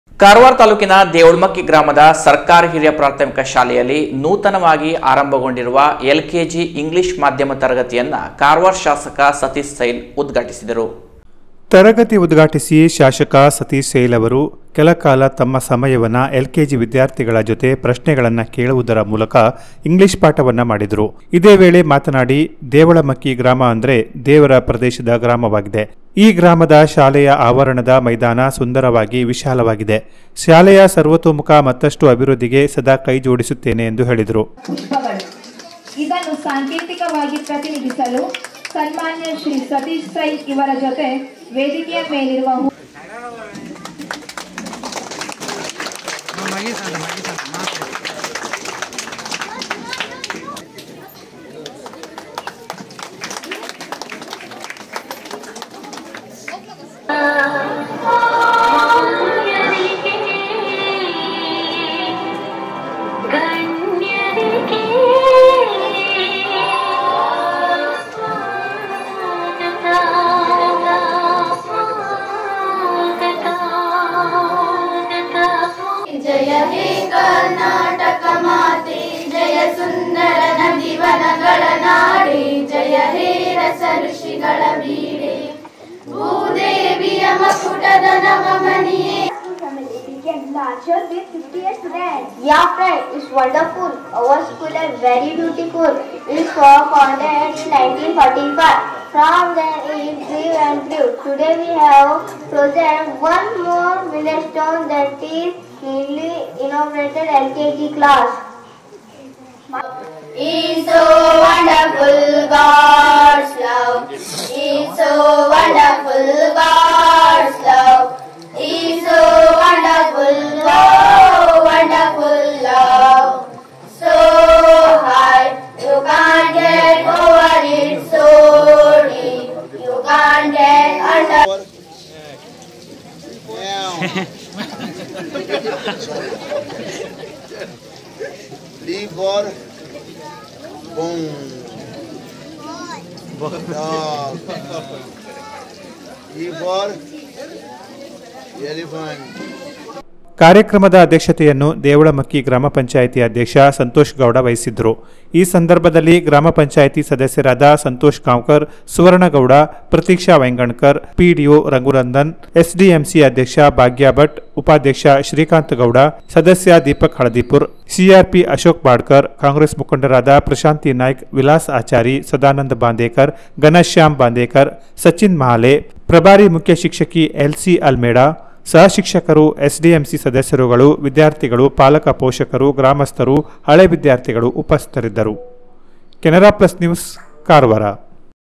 ಎಲ್ ಕೆ ಜಿ ಇಂಗ್ಲಿಷ್ ಮಾಧ್ಯಮದ ತರಗತಿಯನ್ನು ಉದ್ಘಾಟಿಸಿದ ಶಾಸಕ ಸತೀಶ್ ಸೈಲ್
8-8-24 ಕಾರವಾರ ತಾಲೂಕಿನ ದೇವಳಮಕ್ಕಿ ಗ್ರಾಮದ ಸರಕಾರಿ ಹಿರಿಯ ಪ್ರಾಥಮಿಕ ಶಾಲೆಯಲ್ಲಿ  ನೂತನವಾಗಿ ಆರಂಭಗೊಂಡಿರುವ ಎಲ್ ಕೆ ಜಿ ಇಂಗ್ಲಿಷ್ ಮಾಧ್ಯಮದ ತರಗತಿಯನ್ನು ಕಾರವಾರ ಅಂಕೋಲಾ ವಿಧಾನಸಭಾ ಕ್ಷೇತ್ರದ ಶಾಸಕ  ಸತೀಶ್ ಸೈಲ್ ಅವರು ಉದ್ಘಾಟಿಸಿದರು.